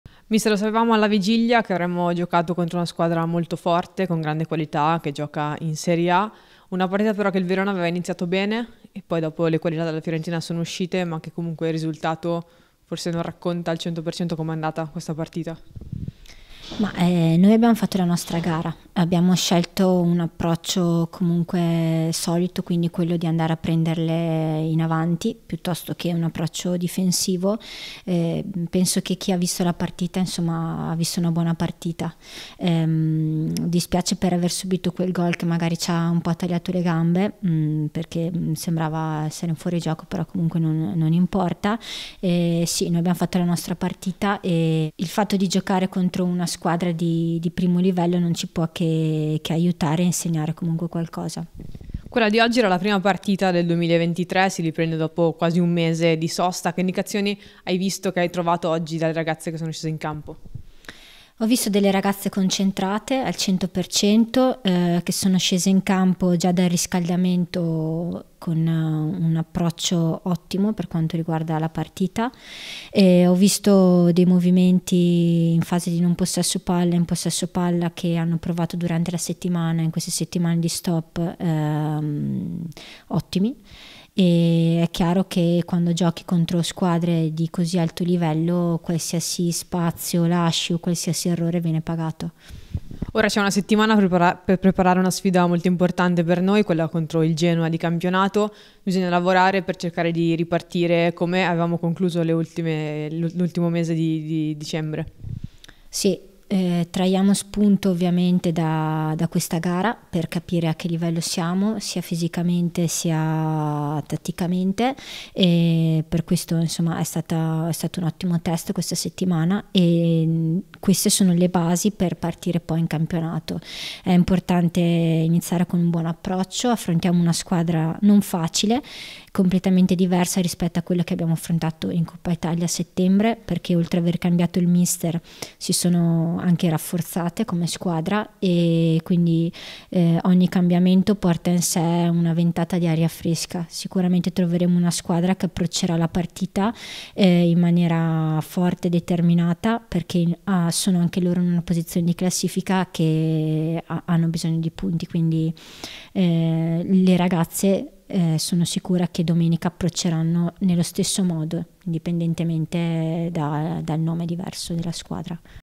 Brutti: "Atteggiamento giusto contro una squadra di categoria superiore, ora pensiamo al campionato"
Le dichiarazioni dell’allenatrice gialloblù Veronica Brutti rilasciate al termine di Hellas Verona-Fiorentina, 3a giornata del Girone E della Coppa Italia Femminile 2022/23.